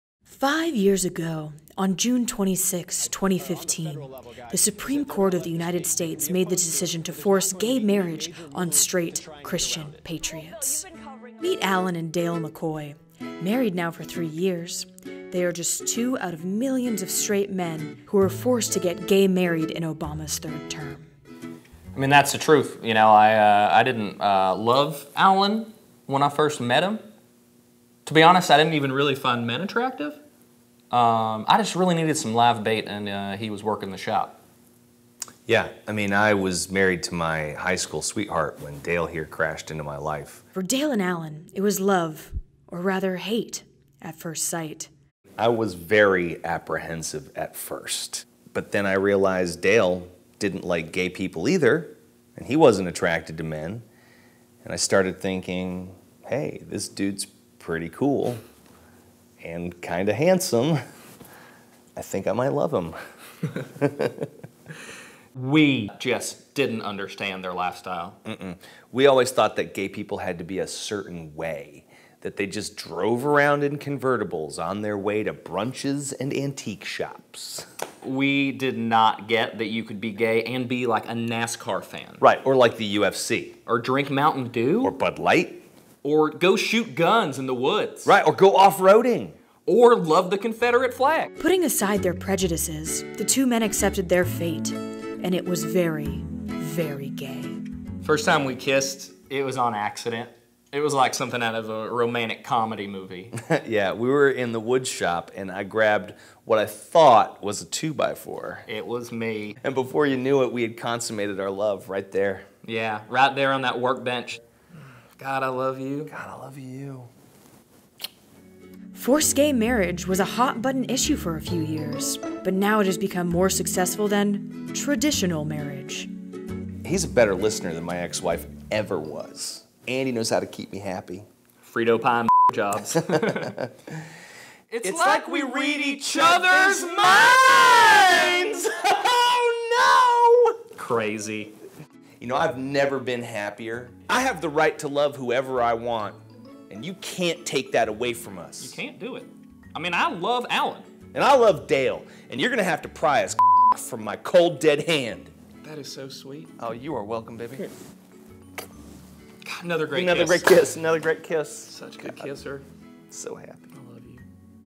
Obama's Third Term - Church Socks Comedy
Obama's Third Term has had quite an impact on straight, Christian Patriots

Starring: Travis Jones as Dale, Ian Covell as Allan
Narrated by: Stephanie Fraser
Directed by: Travis Jones and Stephanie Fraser
Written By: Travis Jones and Ian Covell
Camera & Sound: Jonathon Pawlowski
Editor: Jonathon Pawlowski

Thanks to Highwire Comedy Co. for the collaboration and location

This sketch won the Ladie's Night Competition in Atlanta on 08/06/2015